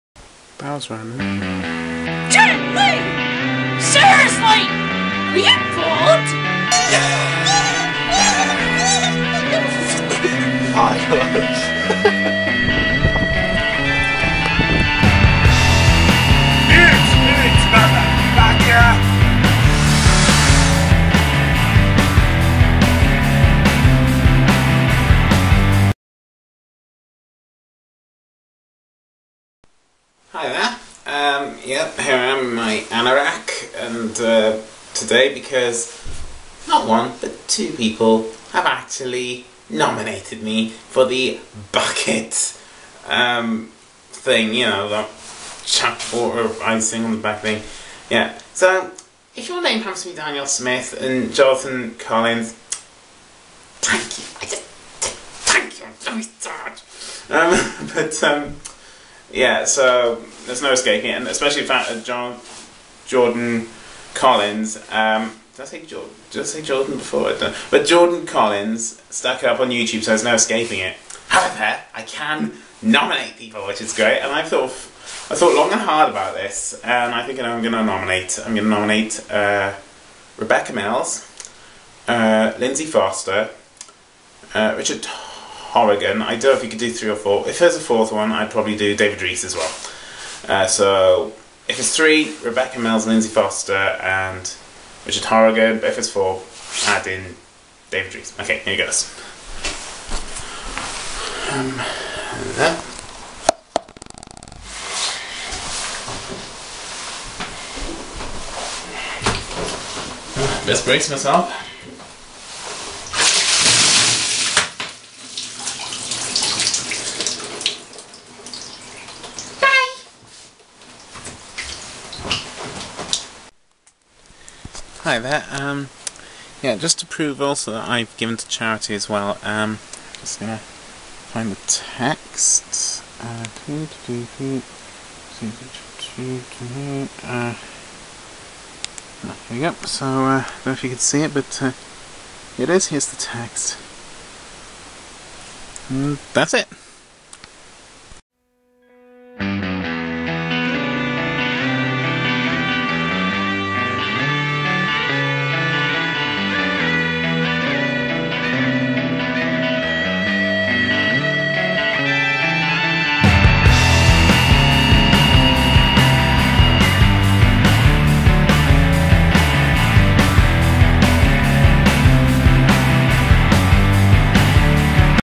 Fit Rich does Ice Bucket Challenge 0001
Well the title says it all. I got nominated by Daniel Smith and Jordan Collins. I nominate Lindsay Gaming Gal Lindsay FosterLindsay Foster ForDrama Richard Horrigan Rebecca Mills and David Rees. Enjoy and as always U RULE :D XXXX